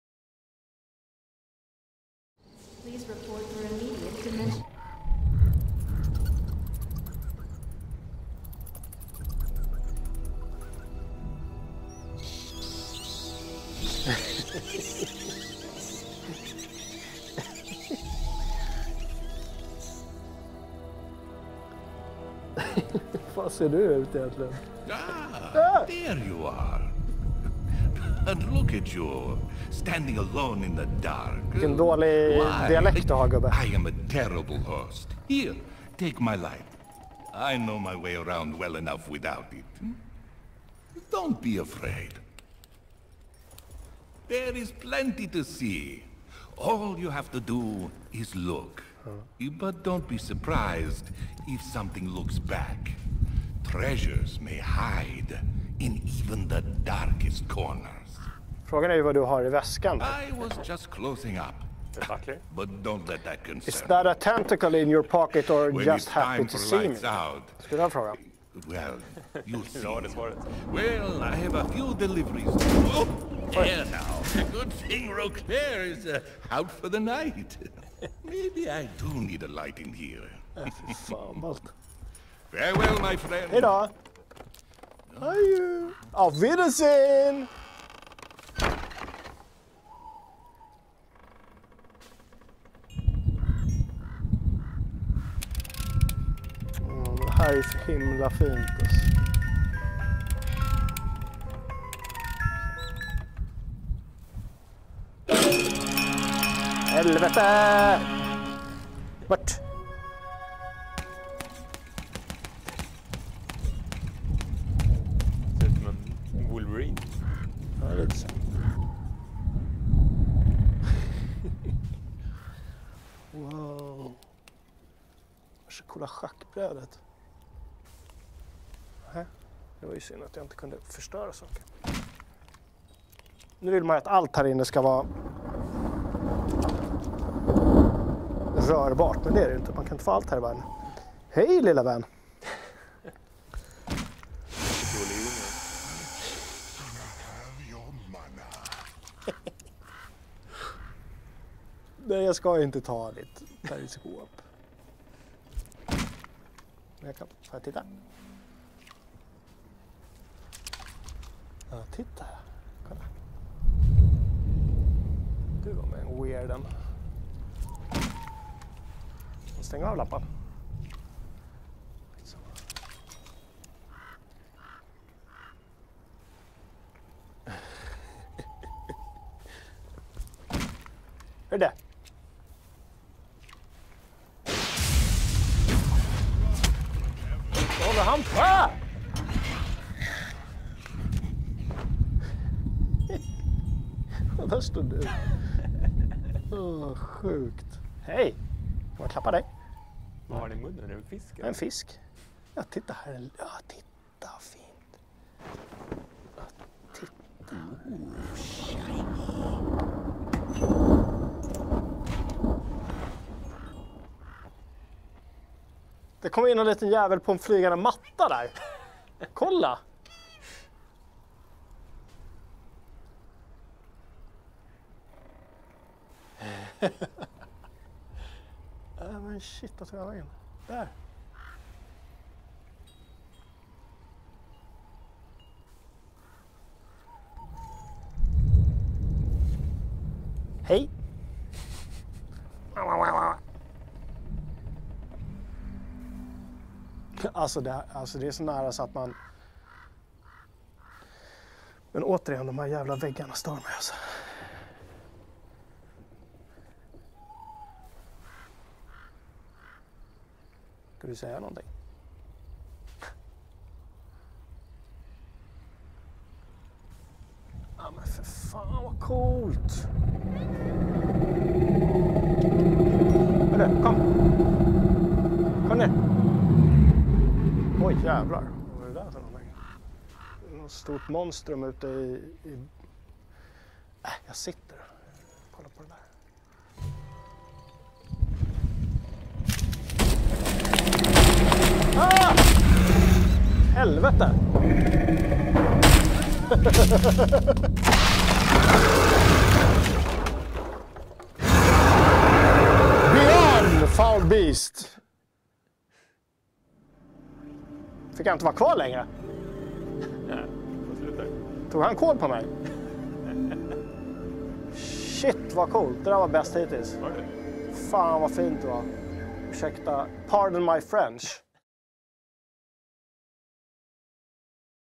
HTC Vive - Jockes VR-favorit - tentakeltroll och blyga bläckfiskar
Det är precis så skruvat som det låter.